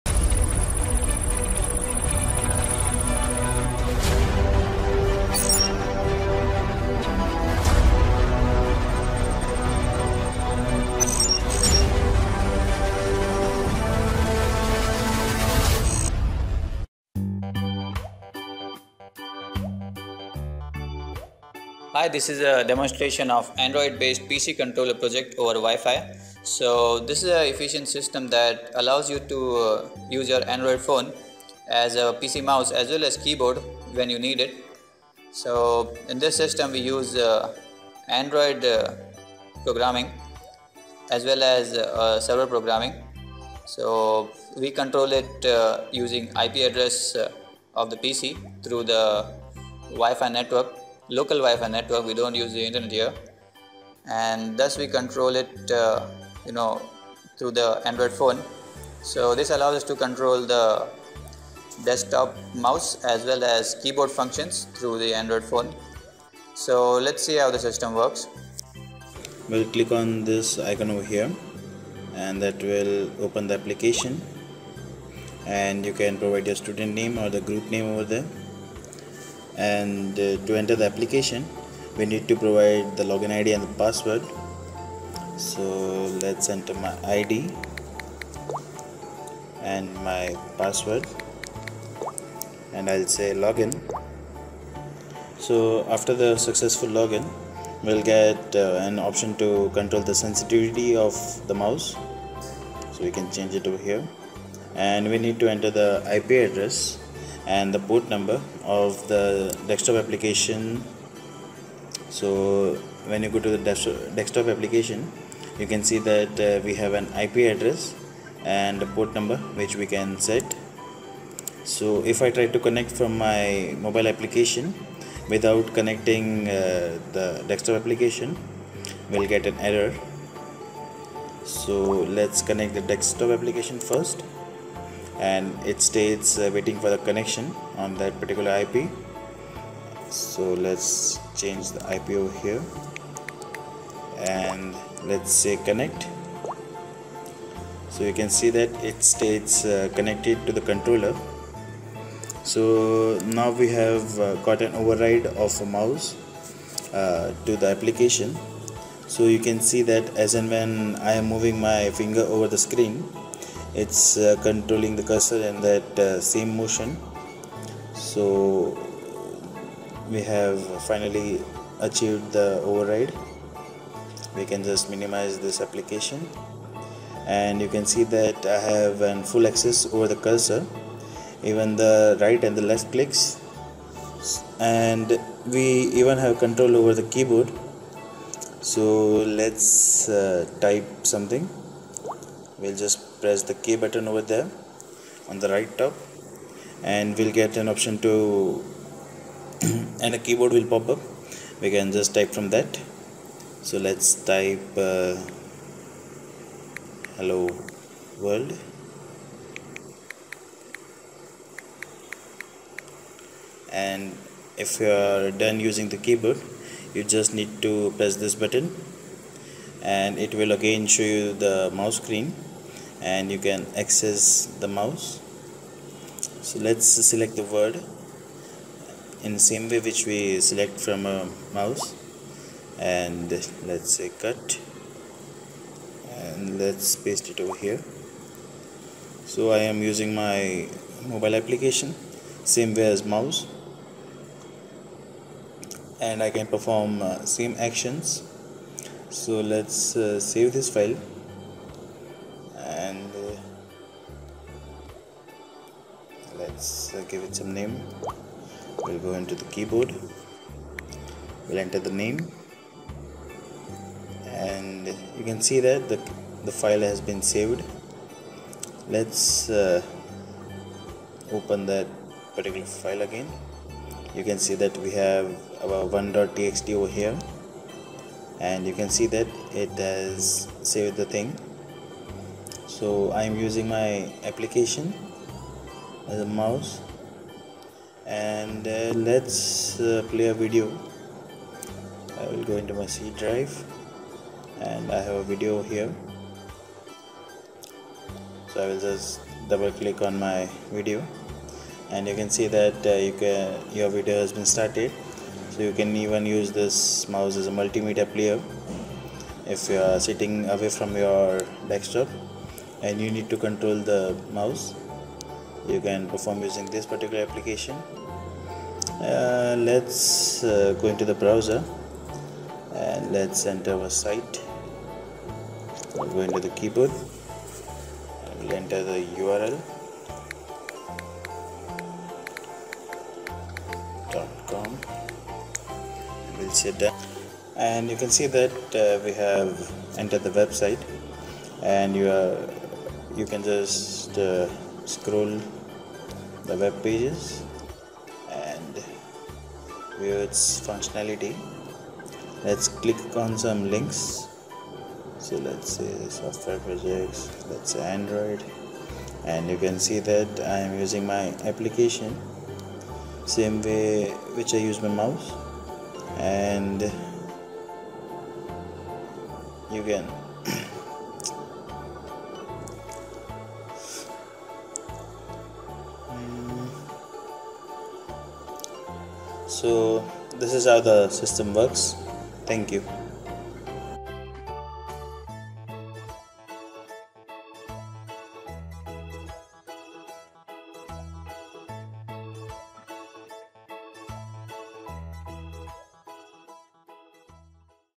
Android Based PC Controller Using Wifi
Android based pc controller project allows user to control pc without internet using local wifi connection for mouse and keyboard data transmission